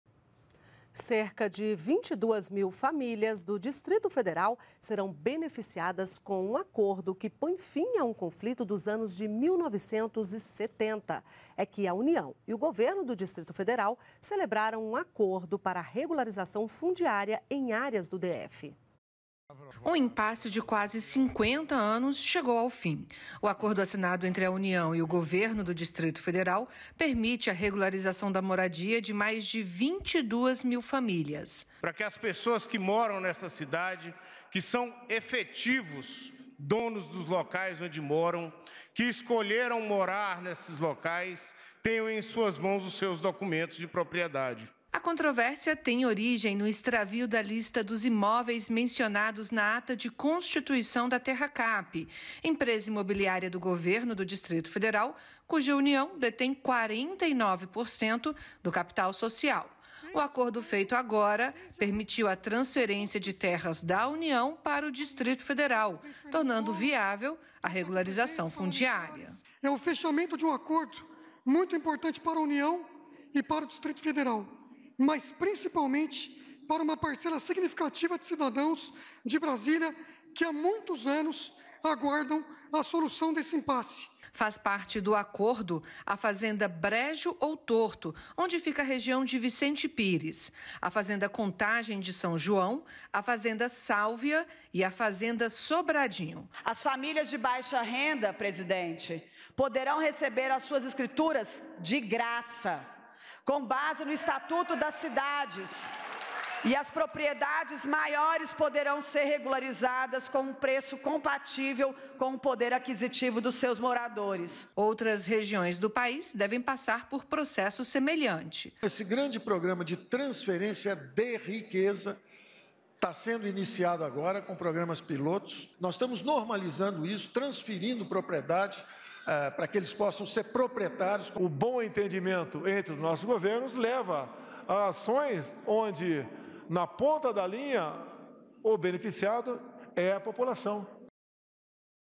Habitação | Regularização fundiária no DF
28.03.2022 - Cerca de 22 mil famílias do Distrito Federal serão beneficiadas com o acordo que põe fim a um conflito dos anos de 1970. É que a União e o governo do Distrito Federal celebraram um acordo para a regularização fundiária em áreas do DF.